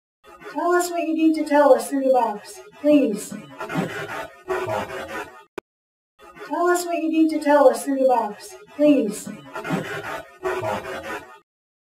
Tell us what you need to tell us, through the box, please.
Investigator states, "Tell us what you need to tell us, through the box, please." Spirit box response,  "I did that. What about for you?" Recorded on October 25, 2023, at the Georgetown Train Station, Georgetown, DE.